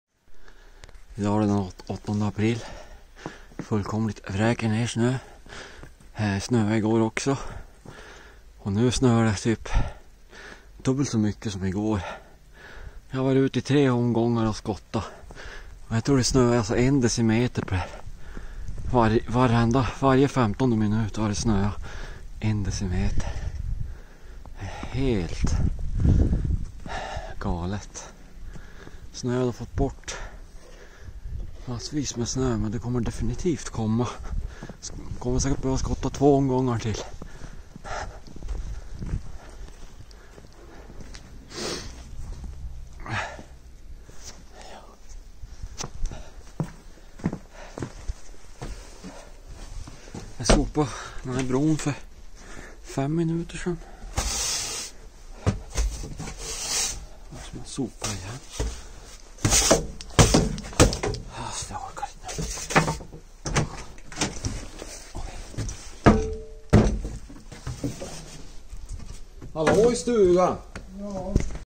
Weather Vlog - Snowing like crazy! (8th April)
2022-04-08 - It is crazy how much snow we have gotten from yesterday until today and it seems to not be stopping. I am here helping my dad shovel away the snow.

Please avoid the smaller roads today if you are up here in the north of Sweden.